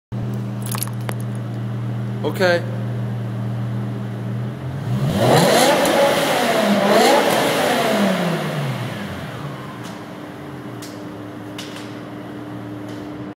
BMW M6 V10 Revving up
BMW E63 M6 S85 V10 revving
Car's warmed up after a long journey from Leicester to central London.
Video doesn't do justice.. sounds much better in real life imo..
This car may have had resonator delete.